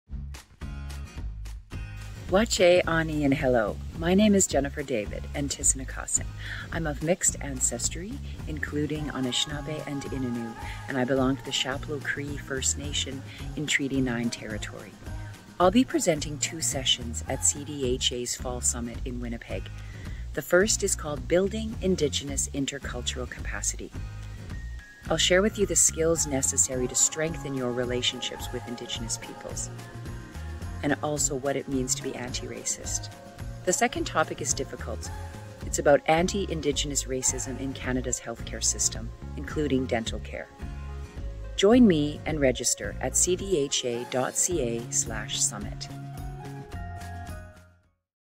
CDHA Summit 2023 Speaker Spotlight: Jennifer David (she/her)
Join us for CDHA's summit Oct. 20–21, 2023, in Winnipeg, Manitoba. You'll hear from an incredible line-up of speakers including Jennifer David (she/her), a senior consultant with a majority-Indigenous-owned company called NVision Insight Group. Her second presentation, which will be offered in person and virtually, discusses the impact that colonialism and anti-Indigenous racism had and continue to have on Indigenous peoples’ health. Register today at cdha.ca/summit